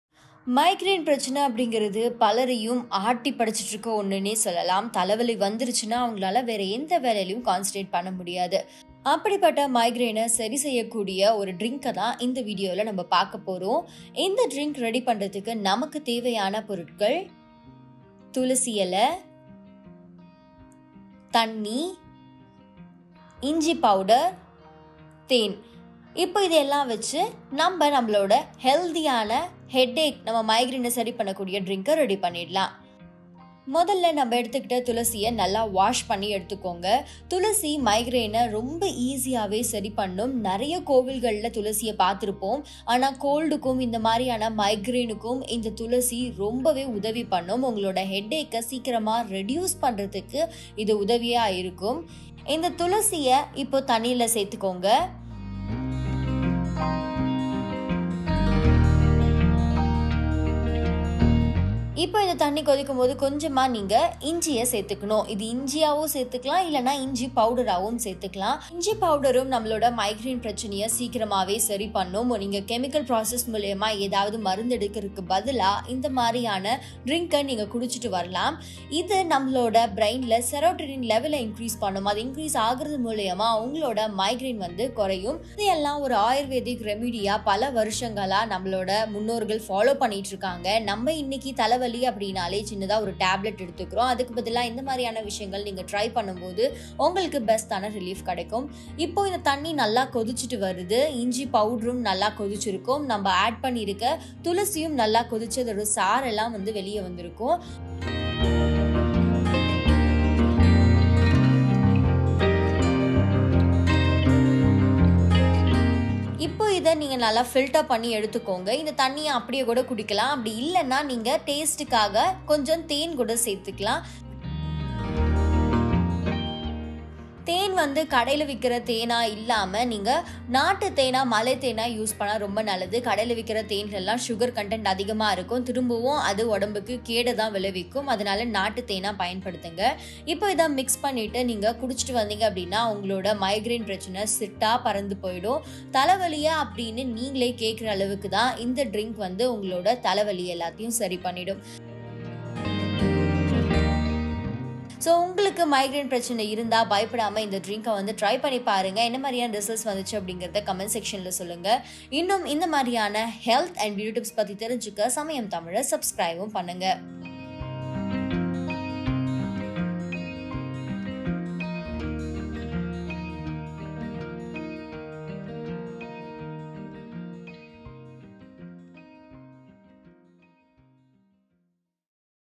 Thalai Vali Kashayam at home | தலைவலி போக்கும் கஷாயம் செய்வது எப்படி? | Samayam Tamil
Migraine attacks aren’t typical headaches. You may experience pounding pain, nausea, and sensitivity to light and sound. When a migraine attack or episode occurs, you’ll do almost anything to make it go away.Now try this home remedy to cure your Migraine ache
தலை வலியை உடனடியாக குணப்படுத்தும் சுவையான ஆரோக்கியமான பானம் வீட்டில் செய்வது எப்படி என்று பார்க்கலாம்